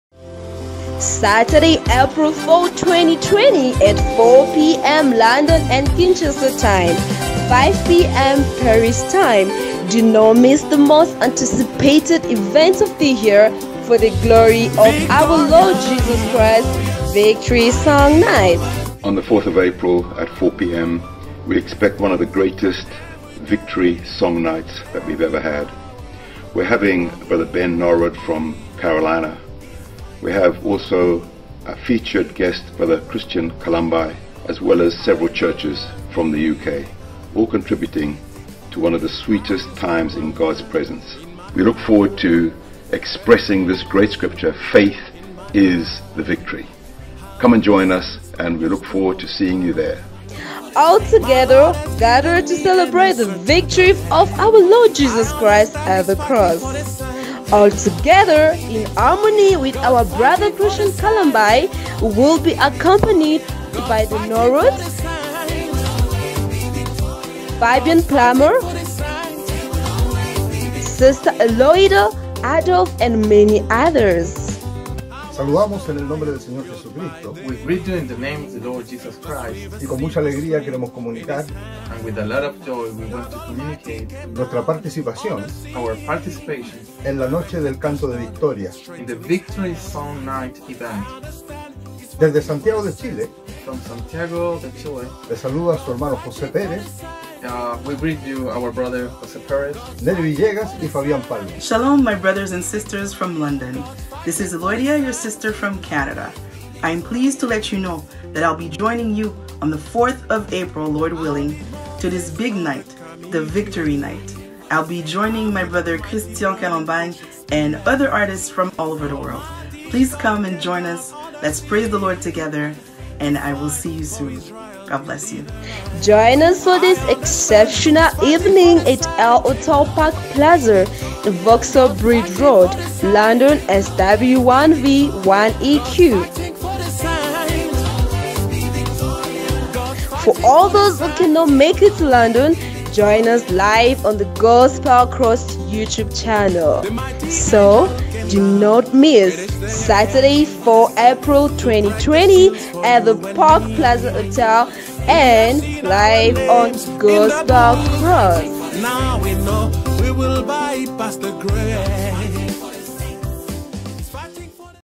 Christian Kalambaie - VICTORY SONG NIGHT - London 04.04.2020 - Concert Advertisement
Saturday April 4, 2020 At 4 p.m. Don't miss the most anticipated event of the year, for the Glory of Our Lord Jesus Christ, Victory Song Night.

All together, gathered to celebrate the Victory of our Lord Jesus Christ at the Cross.

The concert features gifted singers from around the world. Our Brother Christian Kalambaie, The Norrods, Fabian Plama, Sr Eloida Adolphe and many others.

Join us for this exceptional evening at HOTEL PARK PLAZA, 239 Vauxhall Bridge Rd, London SW1V 1EQ

For all those who cannot make it to London, join us live on the GospelCross youtube channel.